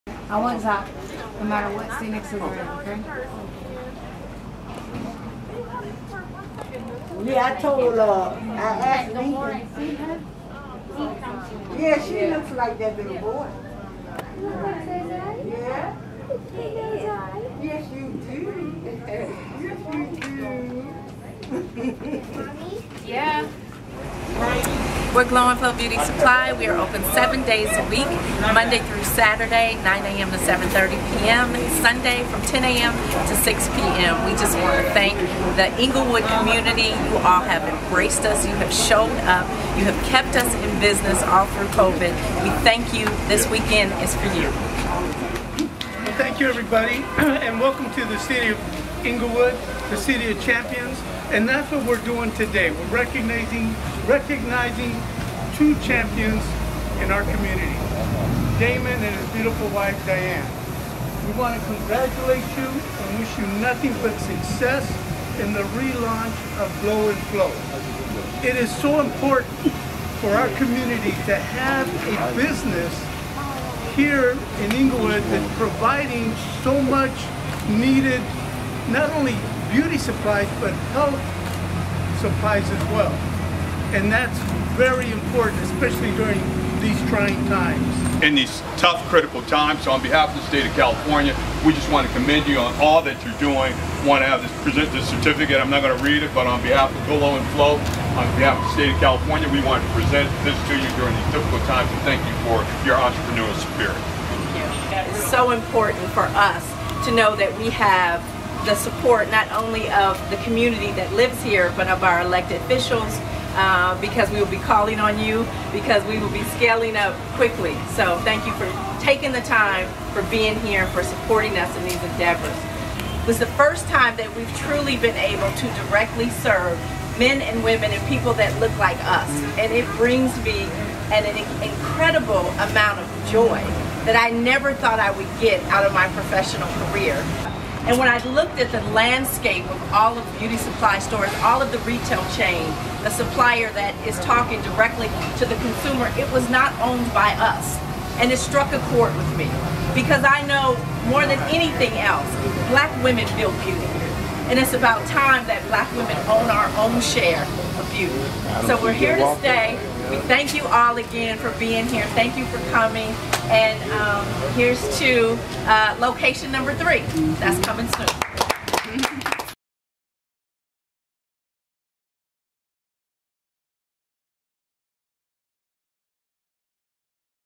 Glow + Flow Beauty Supply Reopening
The Inglewood community celebrates the reopening of Glow + Flow Beauty Supply, previously closed by the COVID-19 pandemic.

Inglewood Times
InglewoodTimes.com